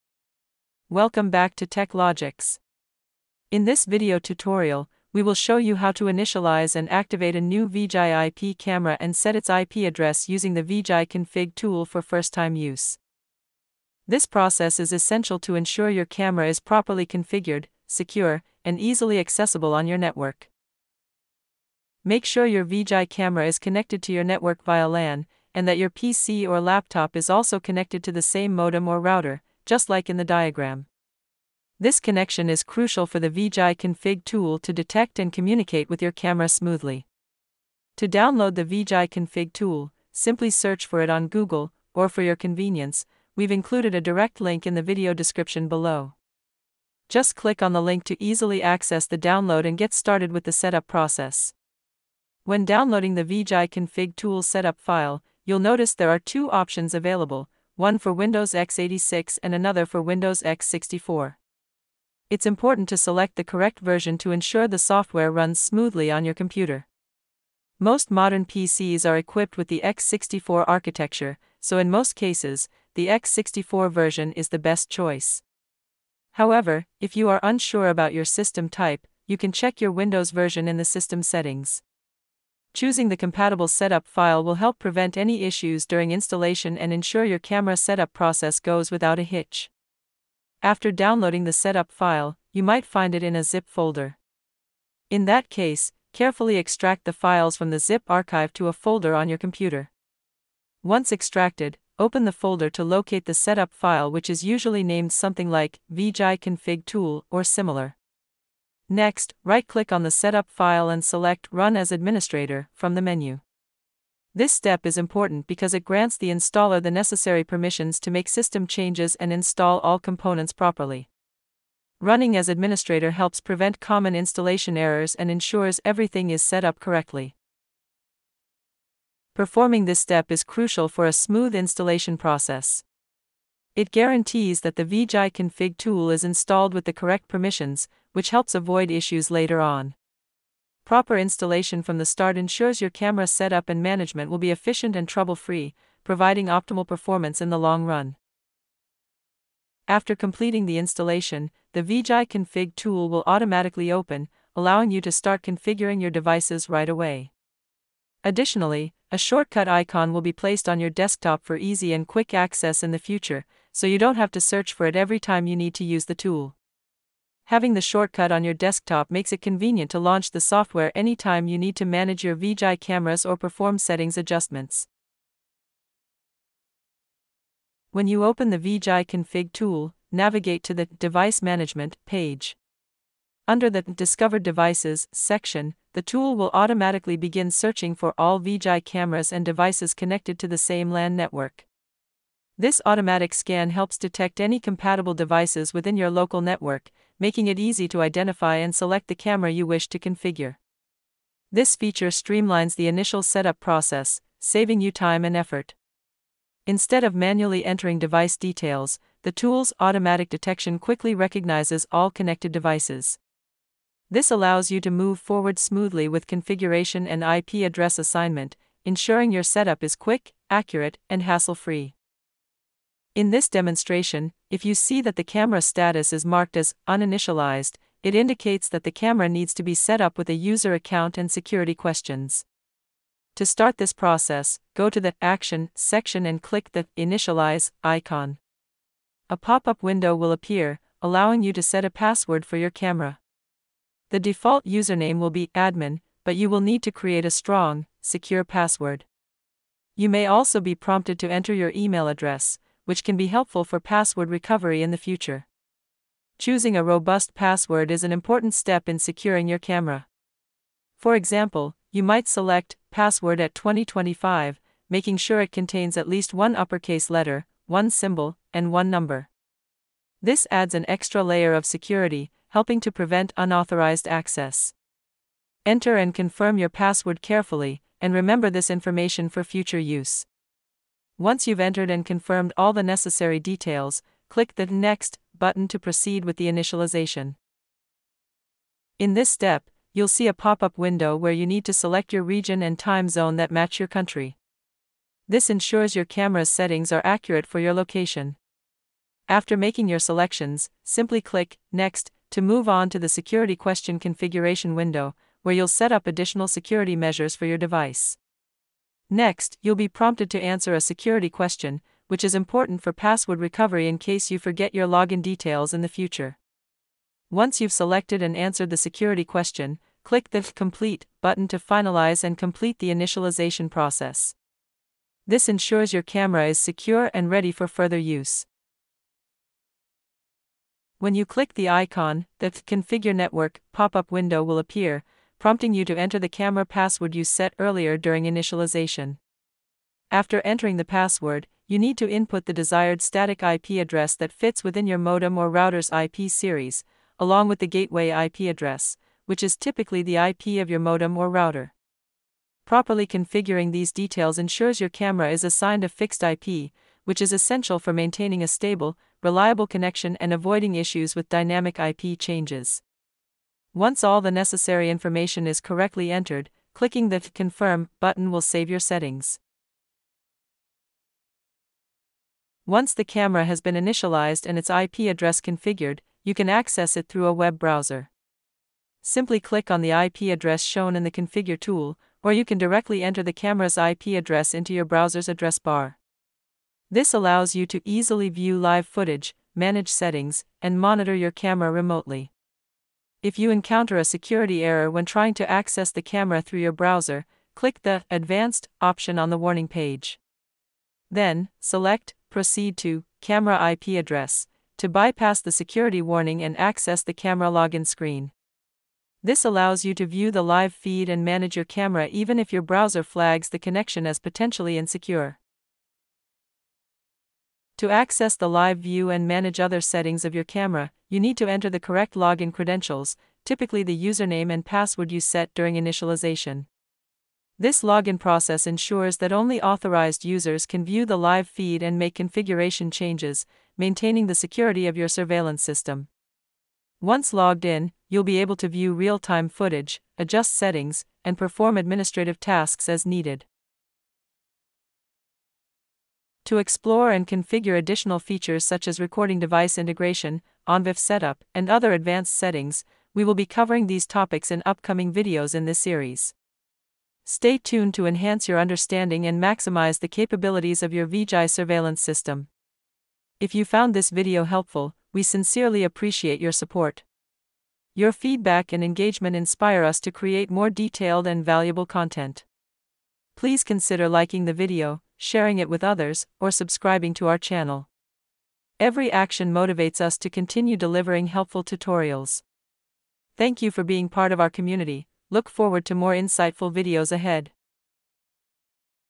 How to Initialize & Set Up IP of Your VIGI IP Camera with VIGI Config Tool | Step-by-Step Tutorial
Welcome back to TECH LOGICS! In this detailed tutorial, we guide you through the process of initializing and activating your new VIGI IP camera and setting its IP address using the VIGI Config Tool. Perfect for first-time users, this step-by-step guide ensures your camera is properly configured, secure, and accessible on your network.

🔍 Check your Windows version: Go to Settings ,  System , About to confirm if your PC is X86 or X64.